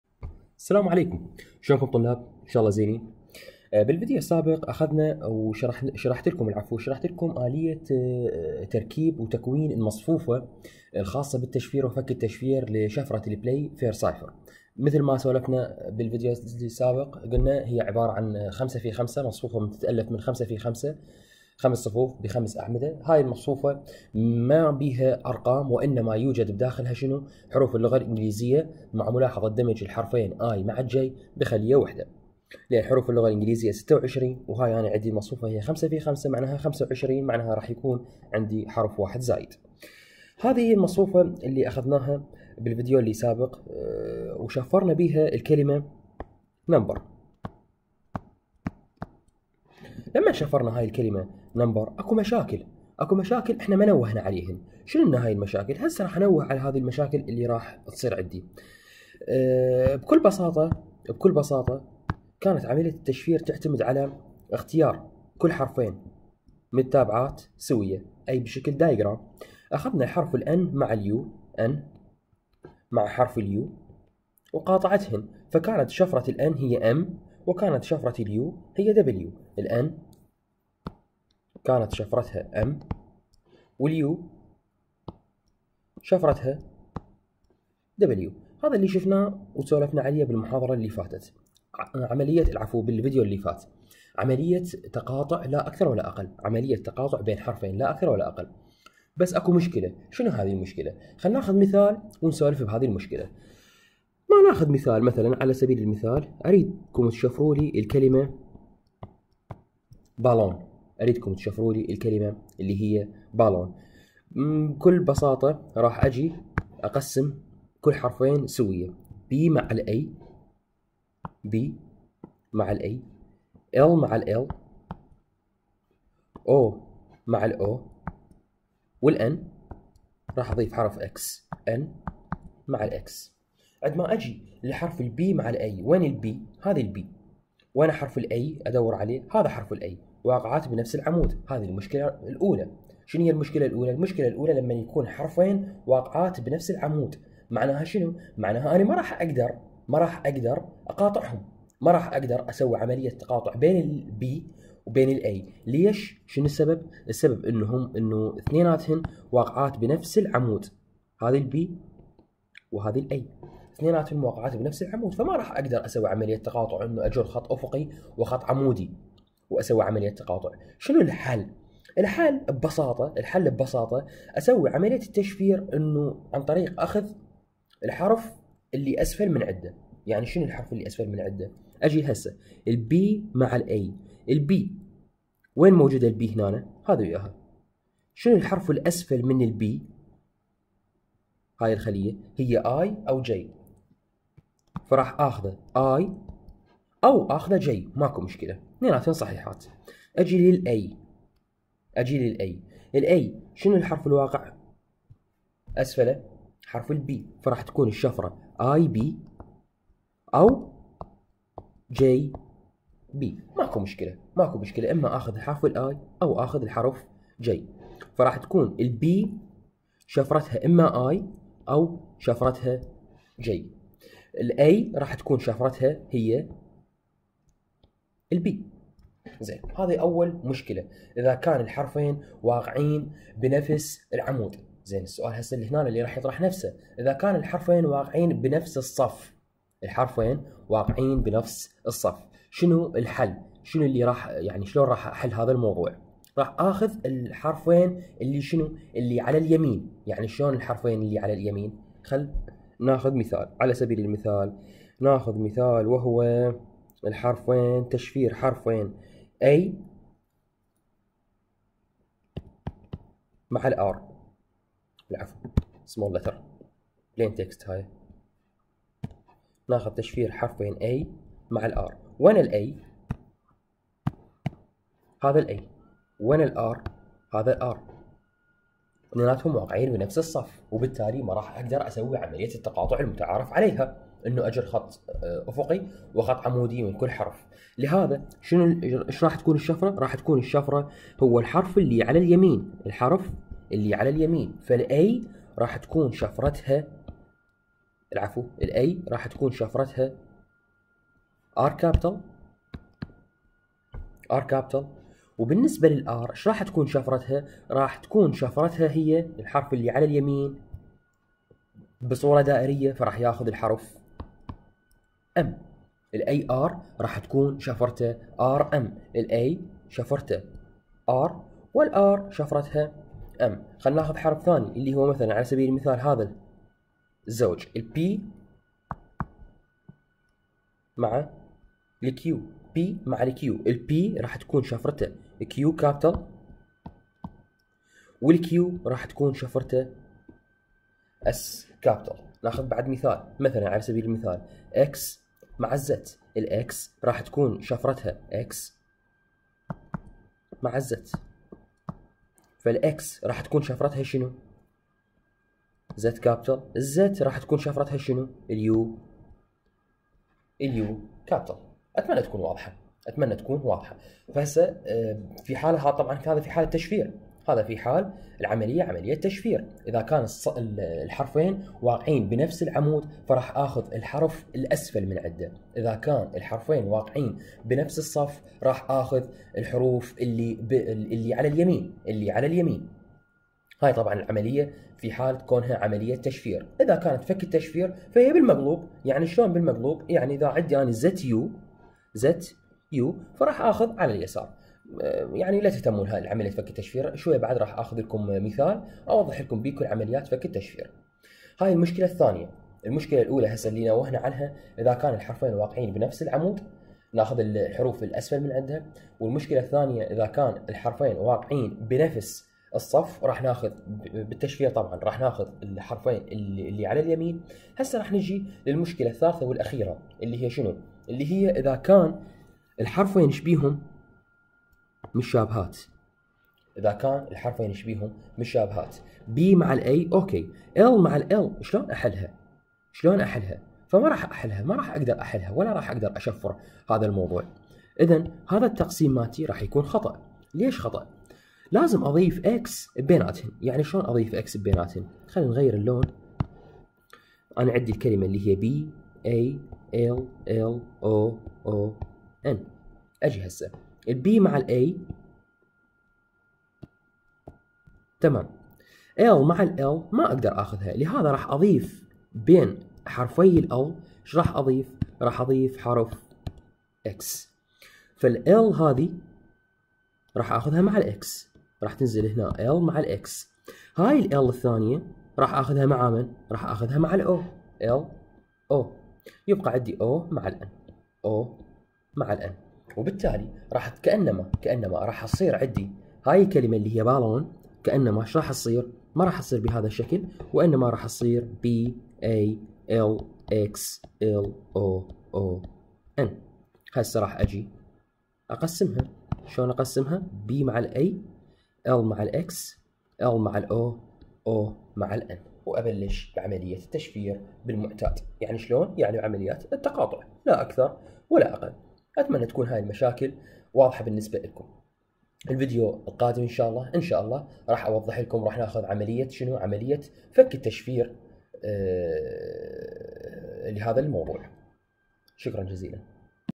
Network Security Lecture/ Playfair Cipher (Part 2)
Cryptographic techniques are the art of converting the plaintext (interpreted text) into ciphertext (uninterpreted text) by using means of letter substitution or/ and transposition.
In this part (part 2), an introduction and principles of the encryption process using playfair cipher is introduced. Playfair cipher is one of the oldest cryptographic techniques that used in the 19th century that relies on the English a letters.